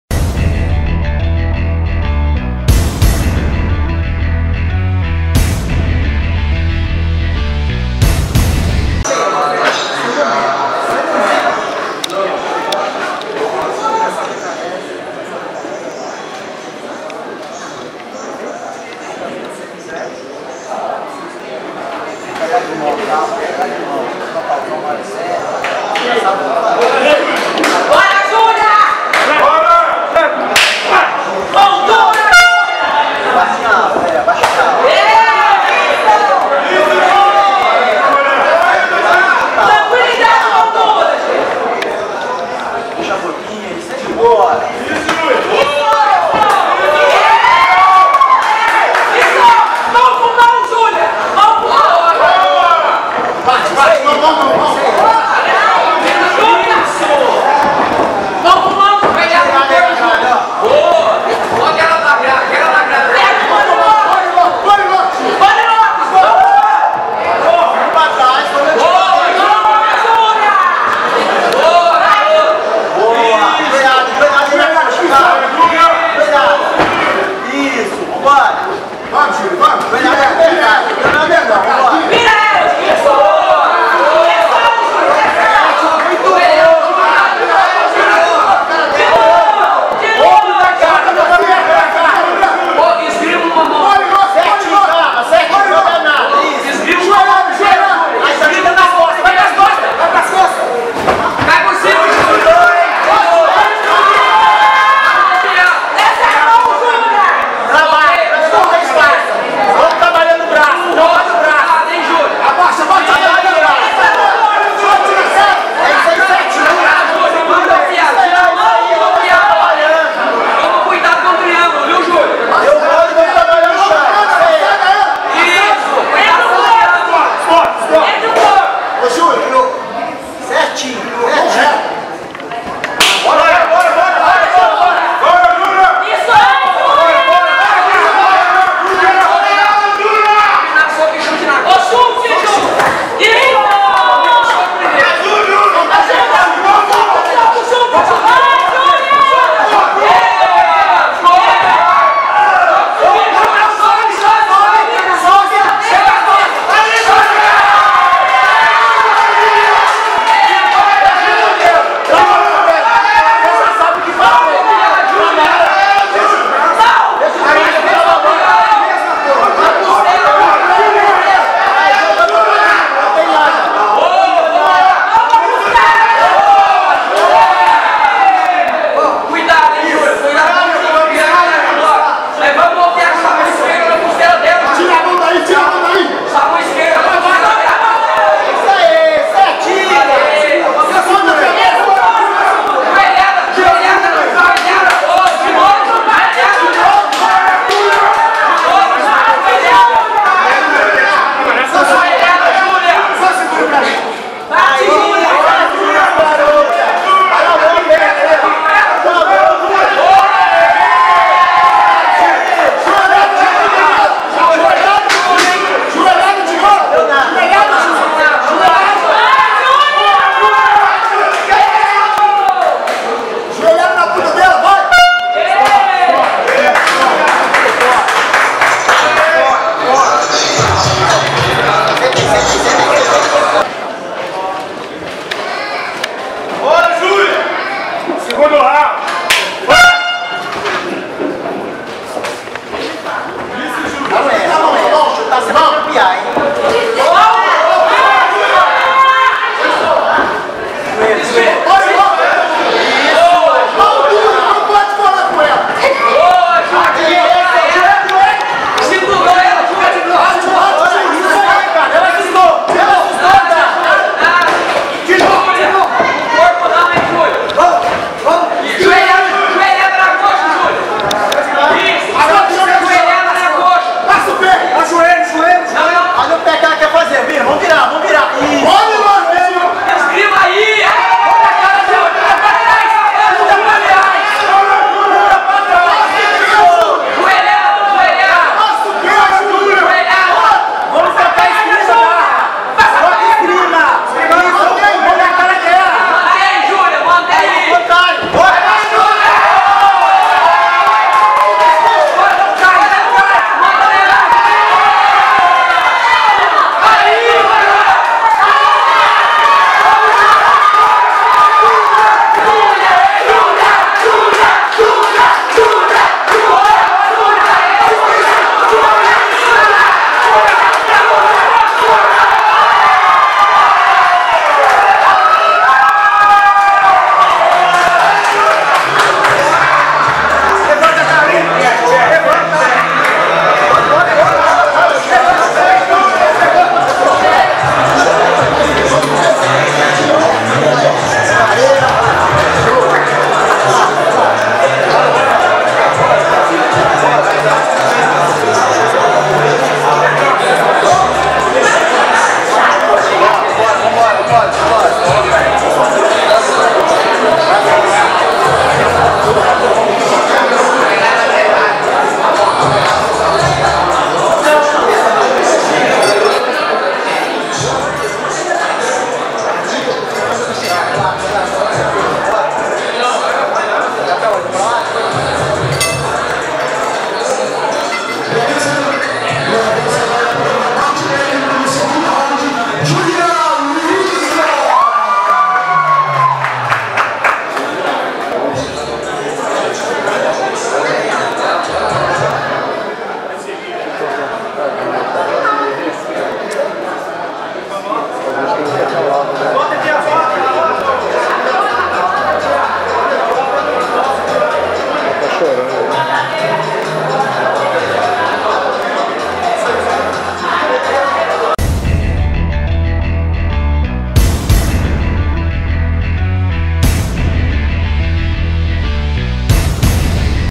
Julia vs Karla - FT Fight Championship 13
Júlia Luisa (Tchaco Fight Team) venceu Karla Yasnara (FTBH) por nocaute técnico no segundo round  52kg.